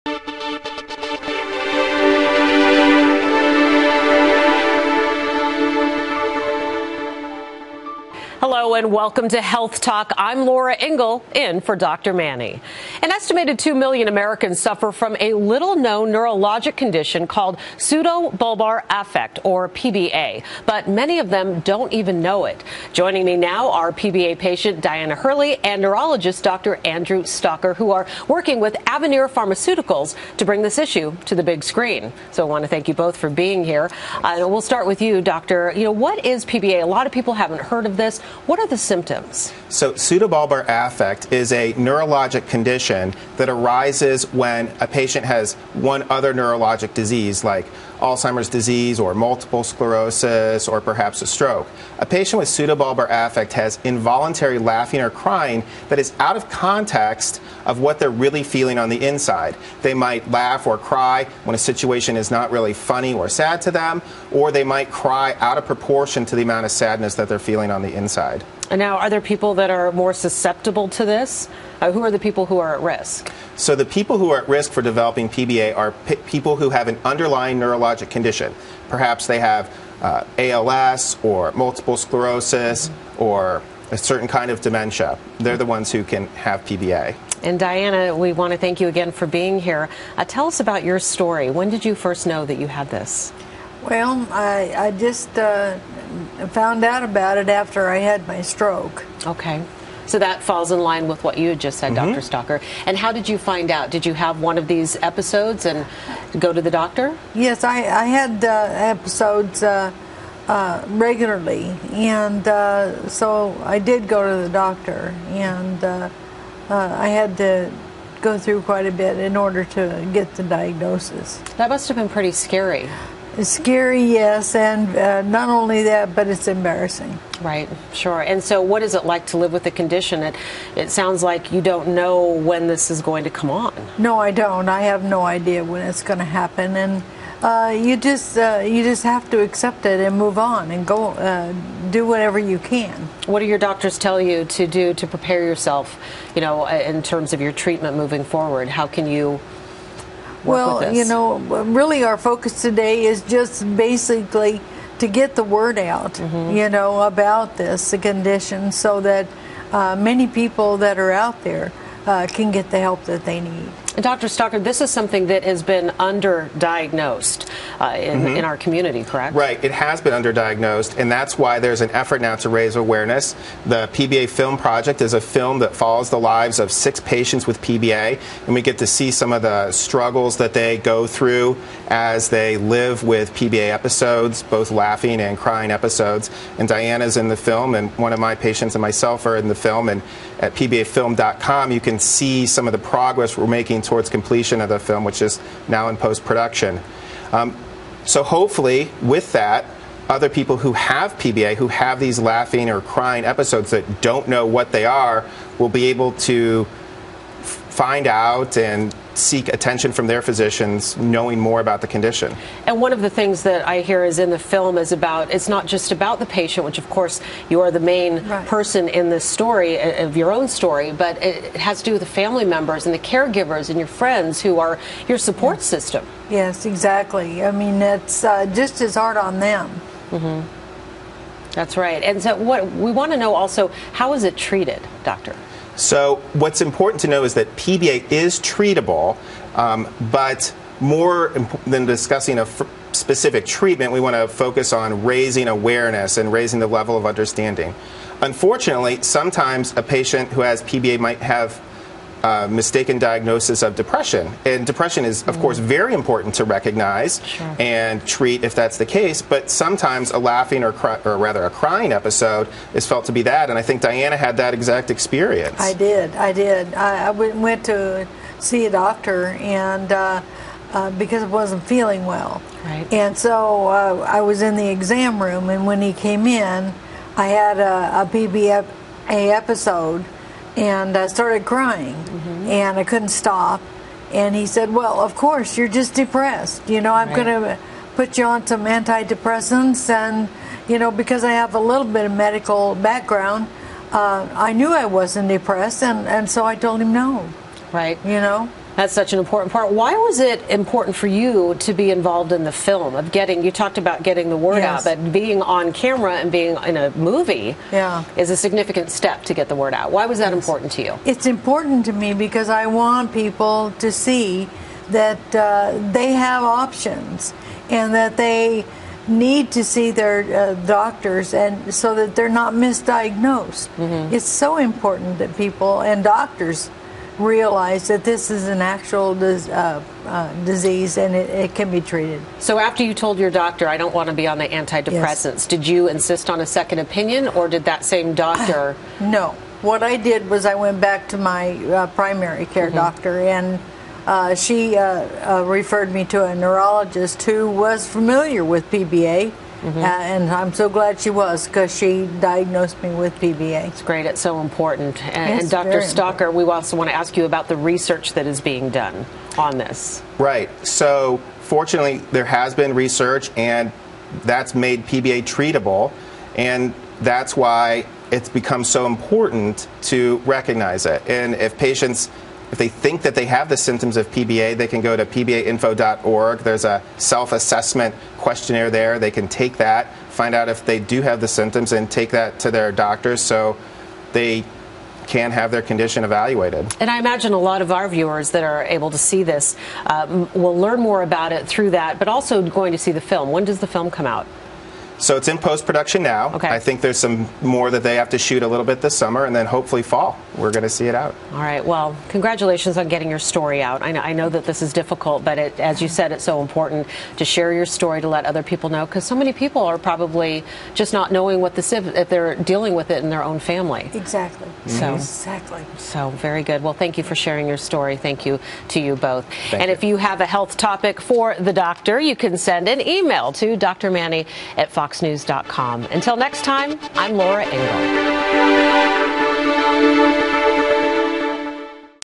Condition causes uncontrollable crying, laughter
An estimated 2 million people suffer from a little-known  neurological condition called pseudobulbar affect (PBA), and many of them don’t even know it. Laura Ingle sits down with a patient and a neurologist who star in a new documentary aimed at shedding light on PBA
Watch Laura Ingle talk about Alzheimers, Head Injury, Health Talk, Multiple Sclerosis, Nervous System, Originals, and Stroke.